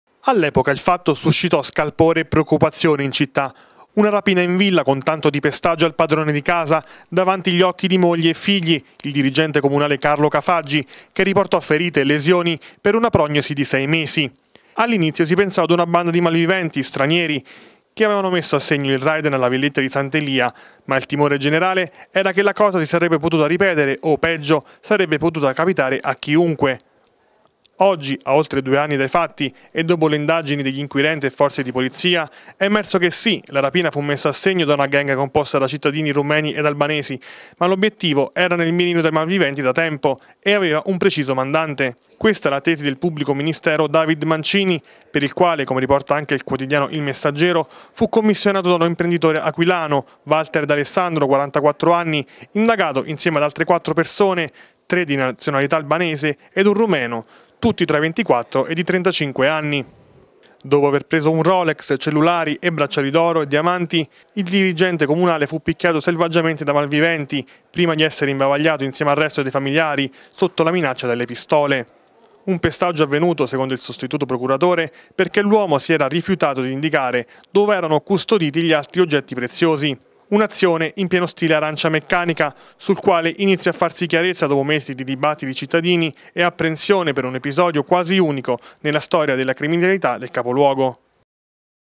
Rapina in villa: c'è il mandante
Per il pm dell'Aquila Mancini sarebbe un imprenditore aquilano il mandante della rapina in villa in cui fu pestato a sangue un dirigente comunale